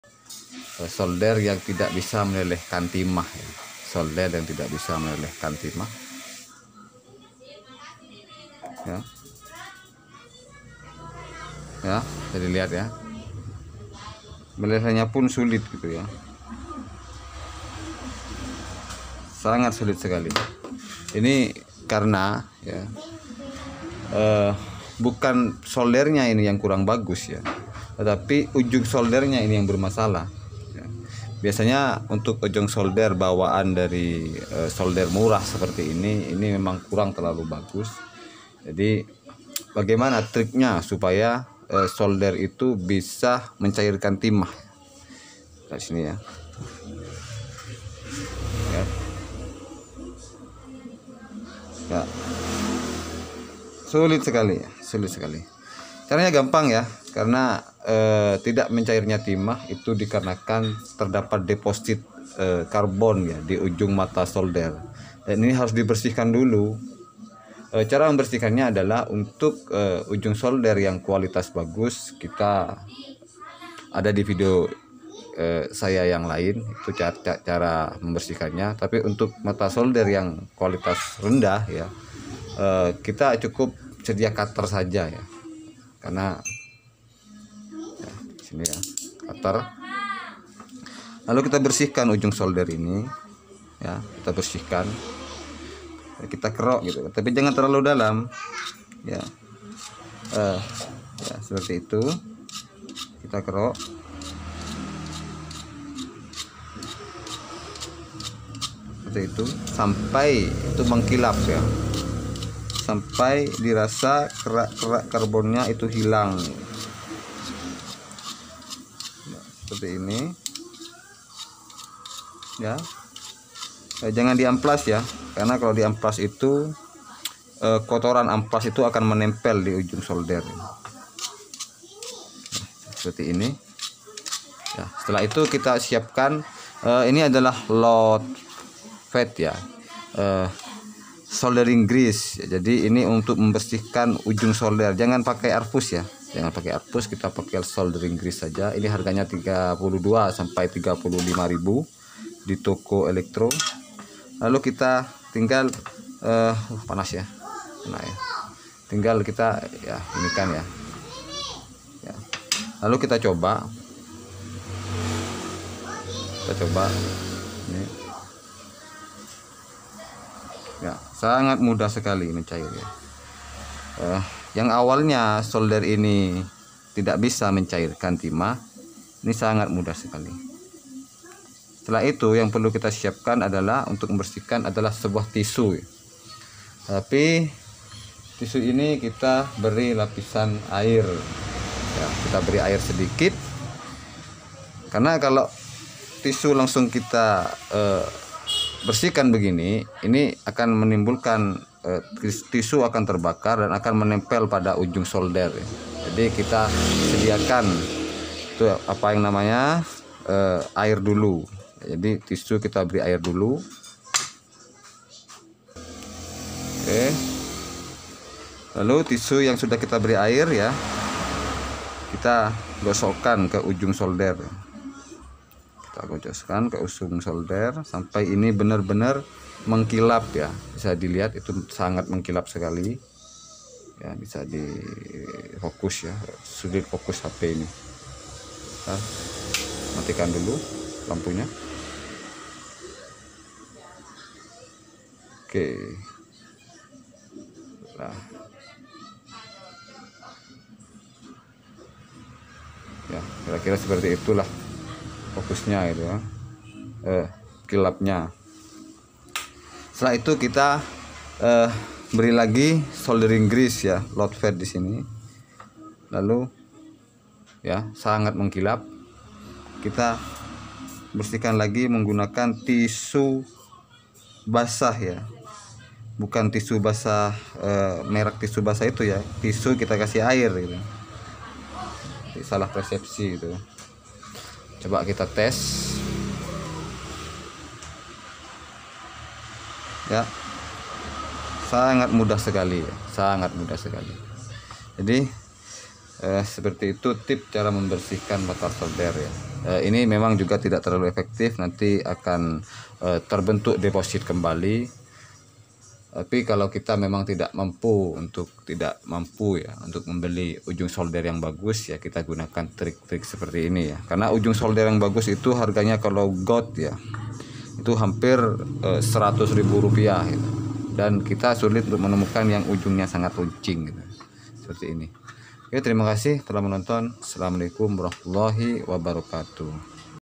Solder tidak bisa melelehkan timah
Solder yang terasa tidak panas disebabkan karena kotoran yang menumpuk pada ujung solder sehingga menyebabkan ujung solder tidak mengkilap dan sulit untuk mencairkan timah.
Masalah tersebut bisa diatasi hanya dengan mengerik bagian yang terlihat kotor lalu diolesi loffet atau soldering grease, agar ujung solder bebas dari kontaminasi kotoran . terutama kerak kerak karbon yang terbakar pada ujung solder.

Dibuat Oleh : dany dwi prayitno ( DH-CELL Bondowoso )

Terimakasih